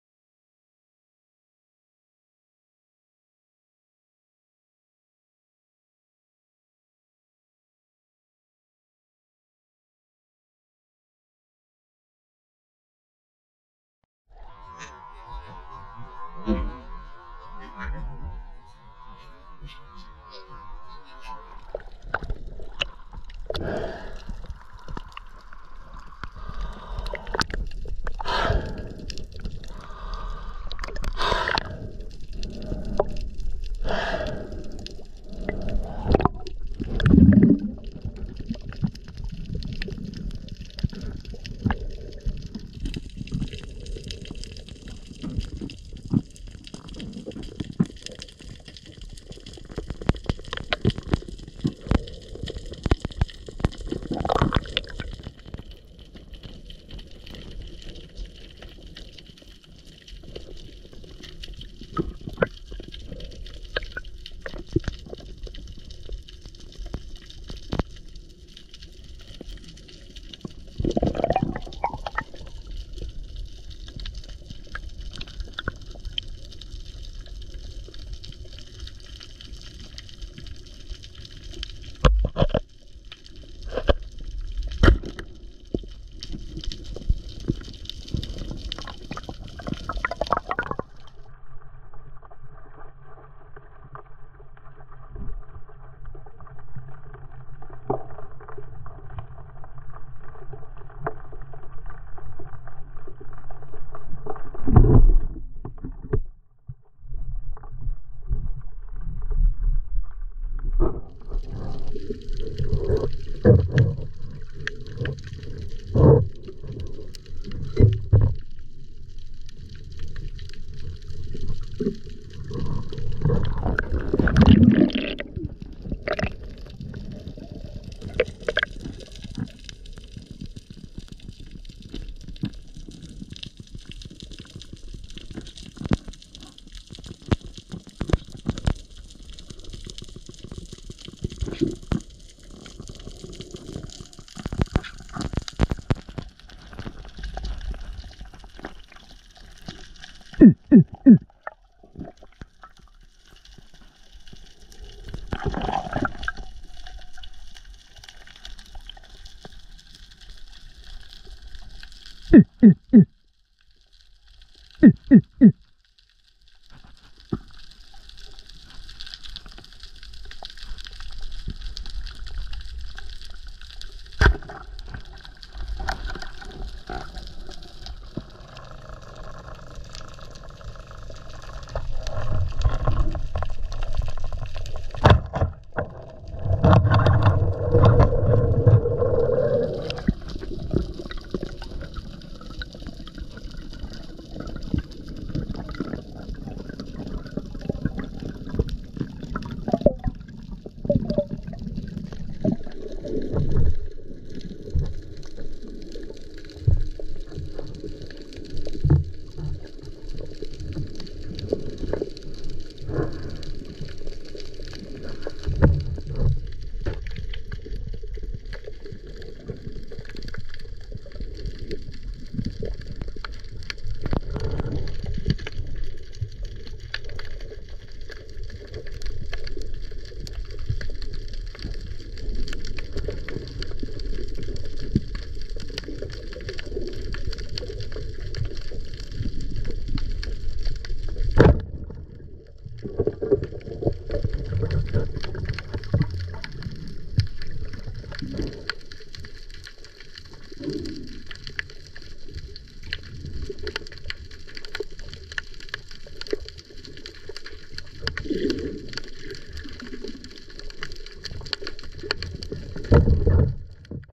Cliff Diving - Spearfishing Malta
In this episode you will see:
00:14 - A warty crab (grottlu in Maltese)
00:23 - A preplanned aguato to reach a white sea bream seen from the surface
02:04 - Aguato for a blue runner
03:37 - Aguato with a white sea bream catch